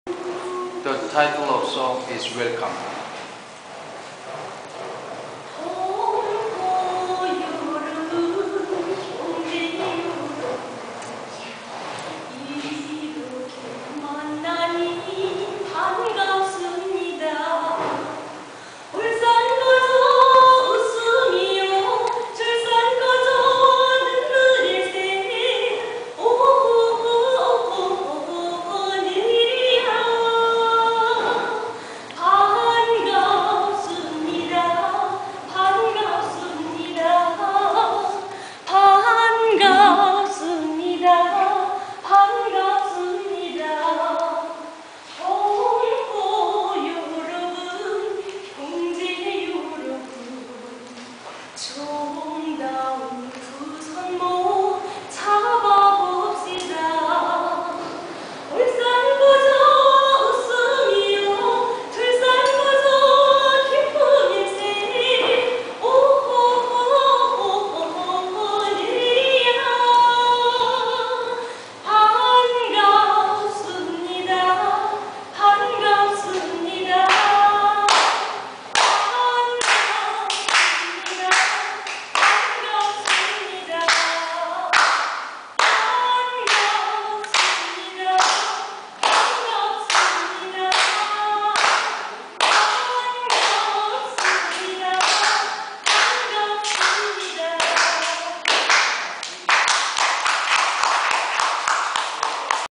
North Korea Popular Song 북한 이미자
AsianInfo.org journeys into mysterious and reclusive North Korea 아름다운 북한 여성의 노래